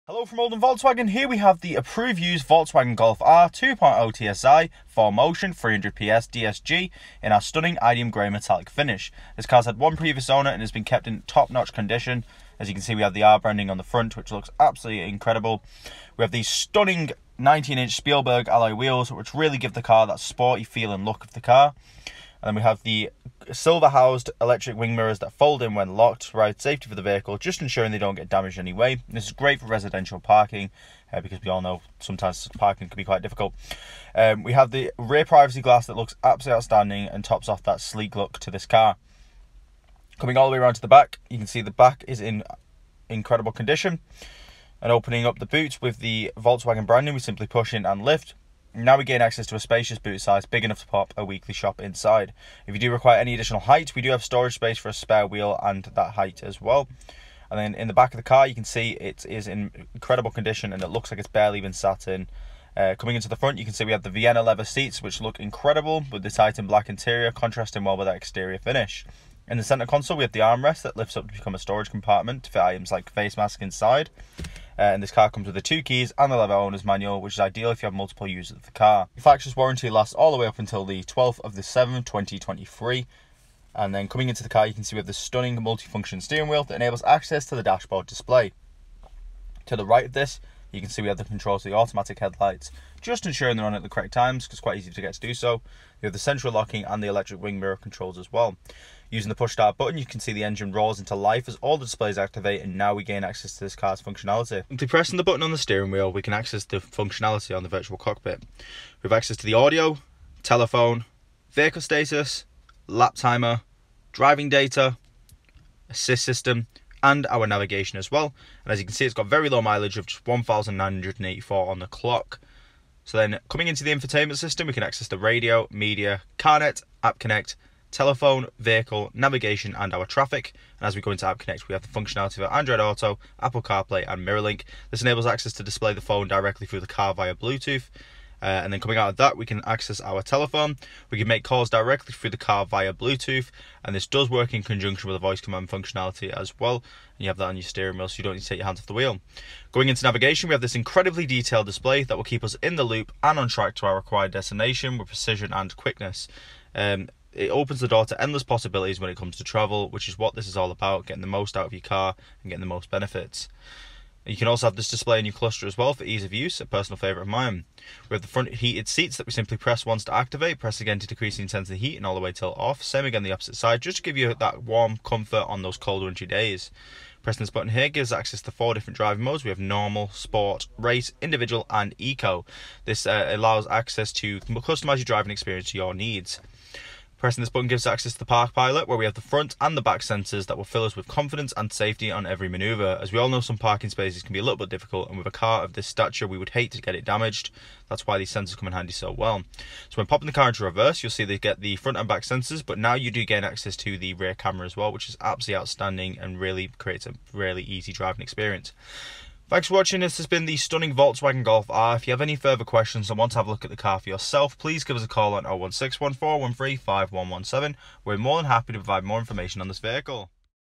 Approved Used Volkswagen Golf R 2.0 TSI 4MOTION 300PS | Oldham Volkswagen
Here we have power and comfort coinciding to create a truly remarkable driving experiences it’s sleek design and sporty interior is enough to turn heads throughout your travels, it comes filled to the brim with brilliant features that will take your driving to the next level.

19inch Spielberg Alloy Wheels

Panoramic Sunroof

Heated seats

Multifunctional steering wheel